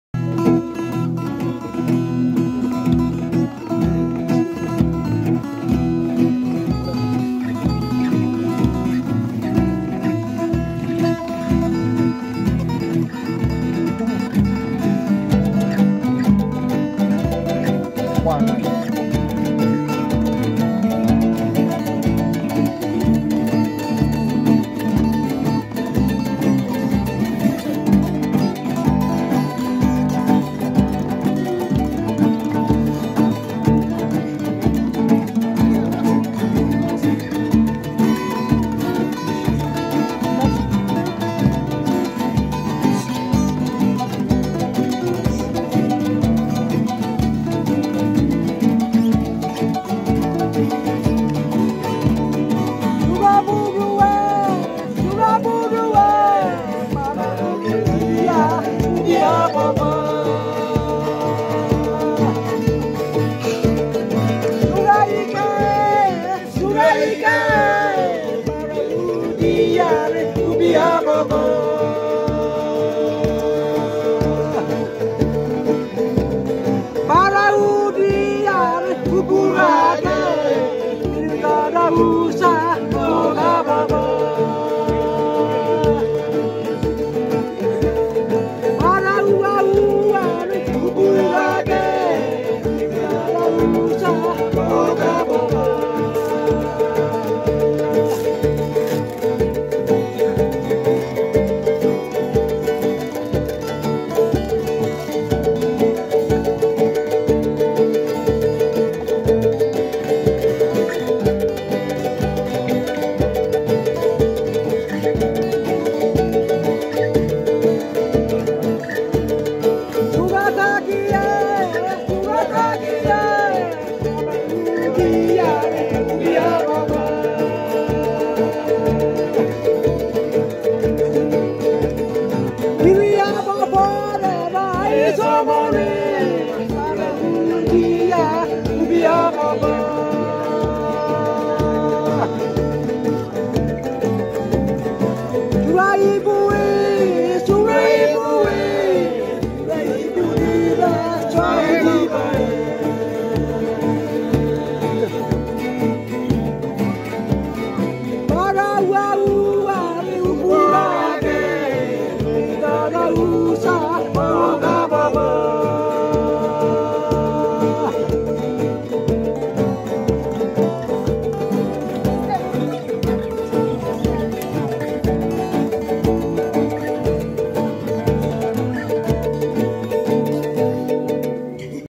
Local talents Kutubu string band :PNG local hits 2023.
The amazing strumming of guitars and ukulele  brings a local contemporary music of PNG an amazing talents.